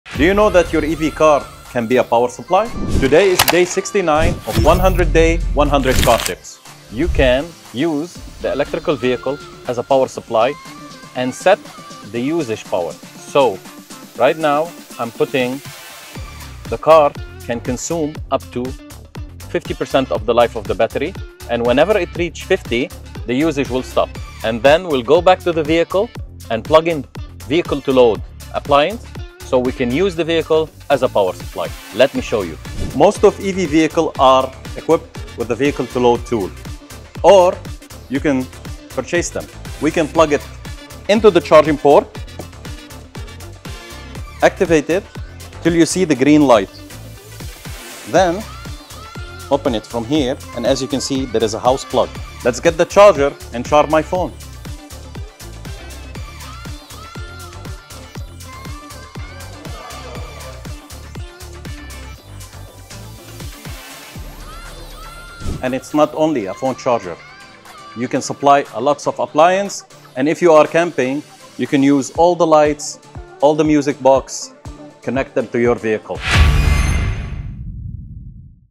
DAY 69: Vehicle-to-Load (V2L) Feature
Vehicle-to-Load turns your EV into a portable power source. You can run tools, charge appliances, or even top up another EV. It is incredibly handy for camping, tailgating, or unexpected outages.